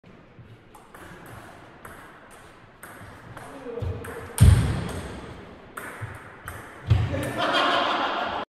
Defying Gravity 🏓🤪🤣🤣
Players: Daniel Gazerani-Lopez & Mark Gee
Venue: Cramlington Table Tennis Club

forehand topspin
backhand topspin
counter topspin